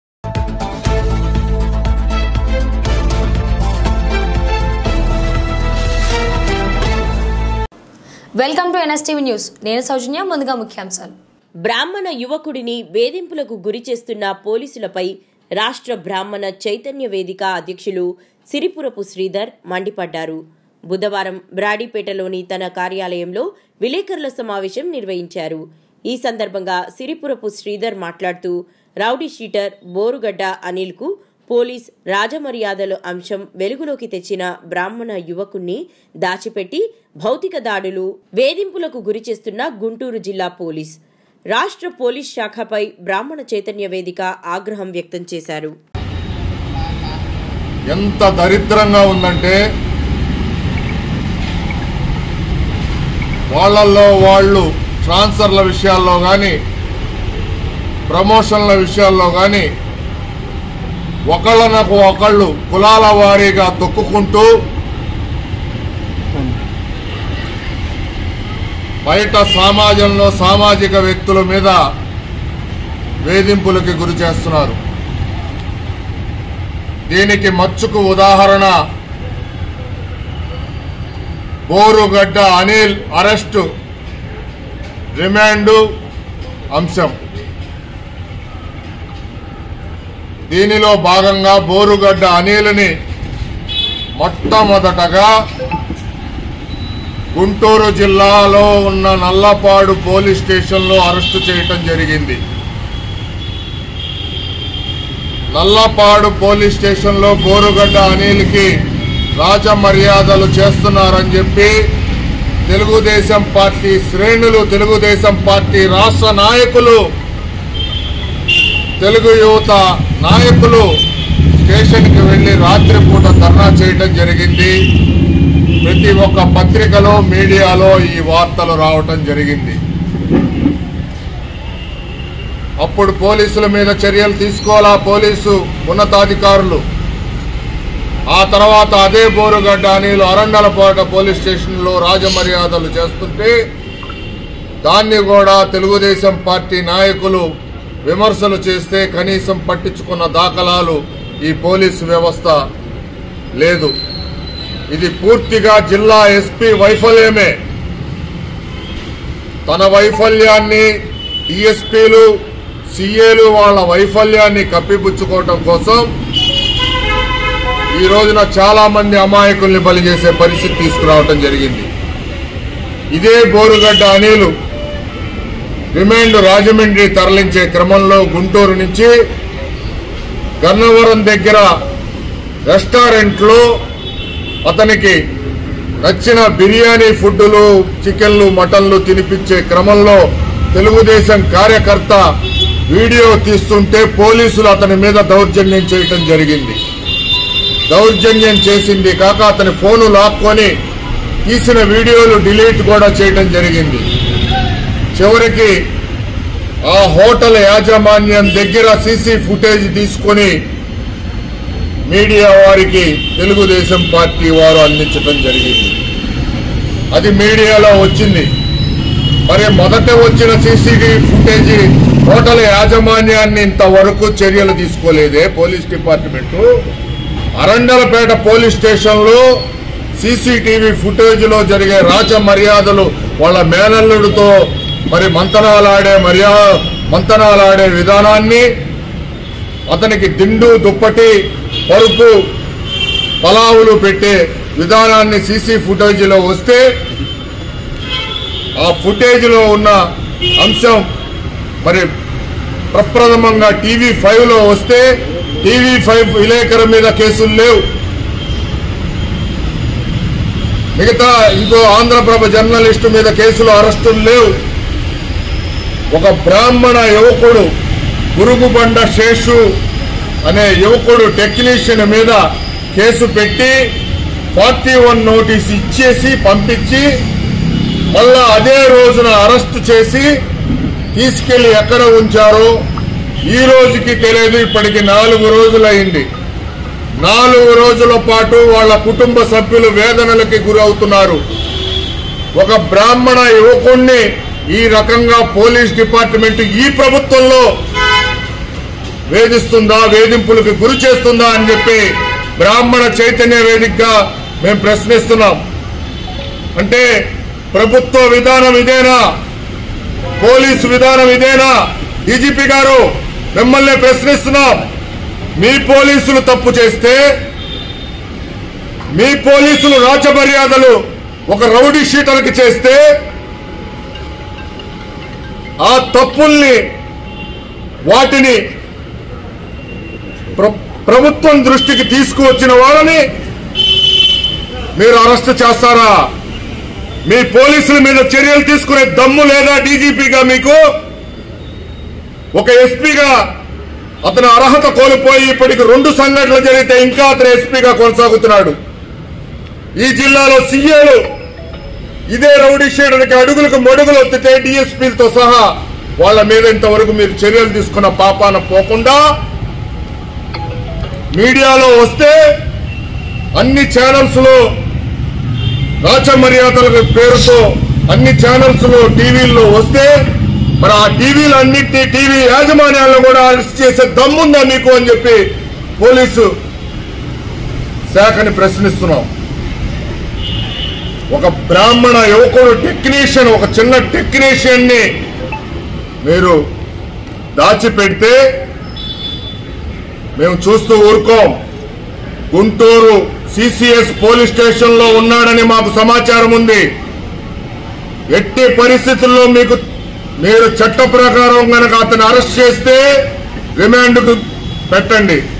బ్రాహ్మణ యువకుడిని వేధింపులకు గురి చేస్తున్న పోలీసులపై మండిపడ్డ - అధ్యక్షులు సిరిపురపు శ్రీధర్
బ్రాహ్మణ యువకుడిని వేధింపులకు గురి చేస్తున్న
 పోలీసులపై  రాష్ట్ర బ్రాహ్మణ చైతన్య వేదిక  అధ్యక్షులు
 సిరిపురపు శ్రీధర్  మండిపడ్డారు.
 బుధవారం బ్రాడీపేట లోని  తన కార్యాలయంలో
 విలేకరుల సమావేశం నిర్వహించారు. ఈ సందర్భంగా
 సిరిపురపు  శ్రీధర్ మాట్లాడుతూ
రౌడీ షీటర్ బోరుగడ్డ అనిల్ కు పోలీస్ రాజ మర్యాదలు అంశం వెలుగులోకి తెచ్చిన  బ్రాహ్మణ యువకున్ని దాచిపెట్టి, భౌతిక దాడులు, వేధింపులకు గురి చేస్తున్న గుంటూరు జిల్లా పోలీస్, రాష్ట్ర పోలీస్ శాఖ పై బ్రాహ్మణ చైతన్య వేదిక ఆగ్రహం వ్యక్తం చేశారు. || @NSTVlive